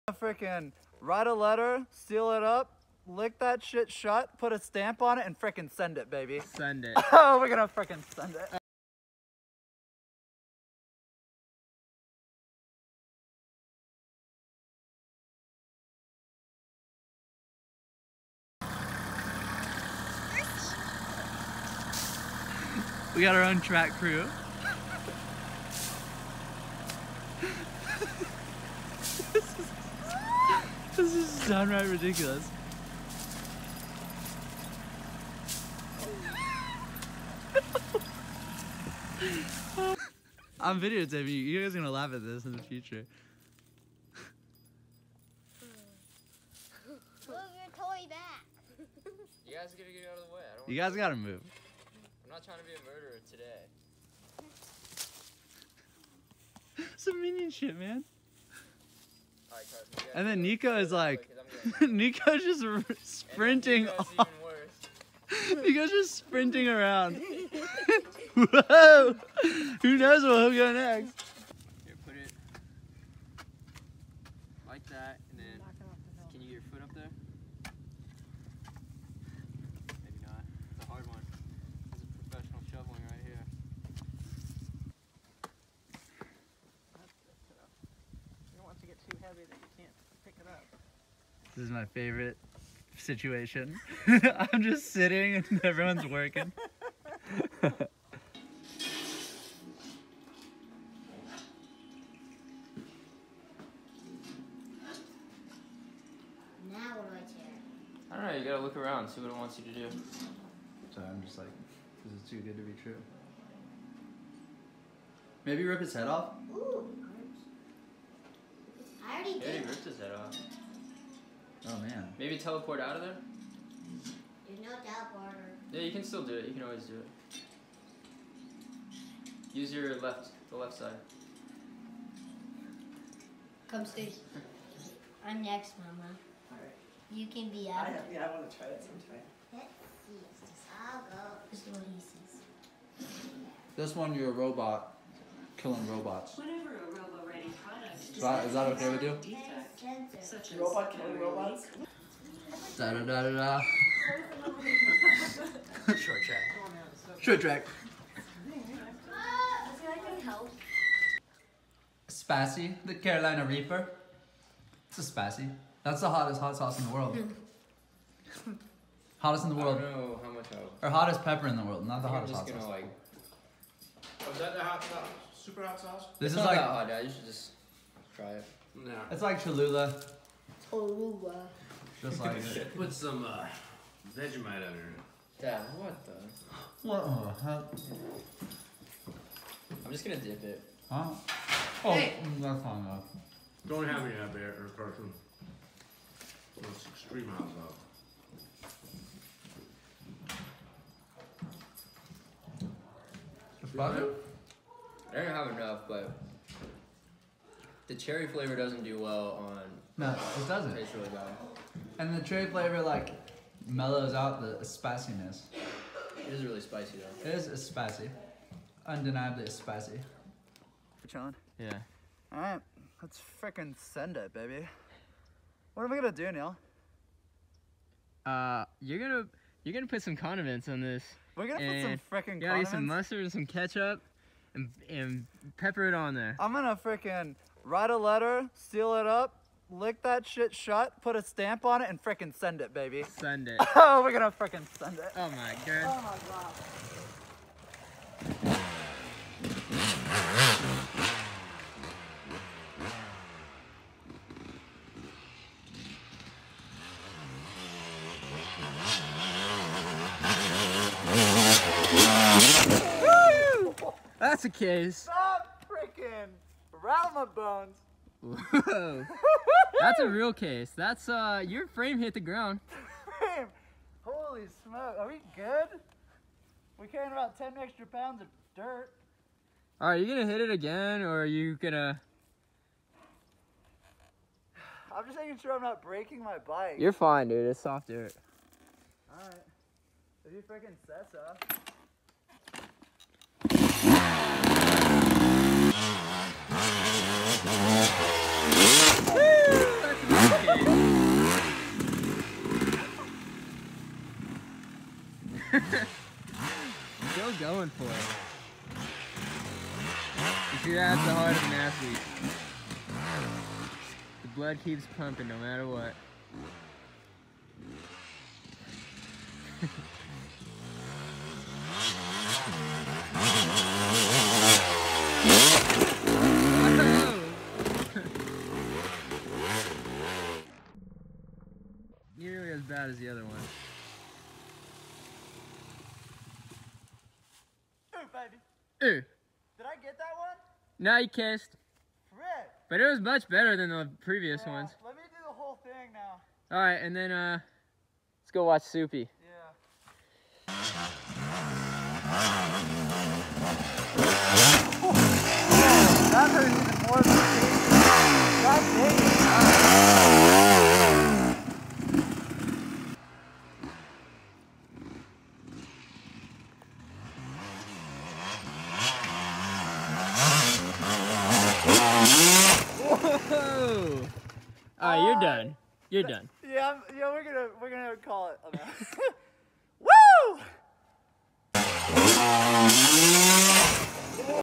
Hardest Motorcycle Case EVER
We put our cousins to work and and I cased really hard.

Business IG: senorsnipes
personal IG: I_love_yoga_pants

HOBBIES?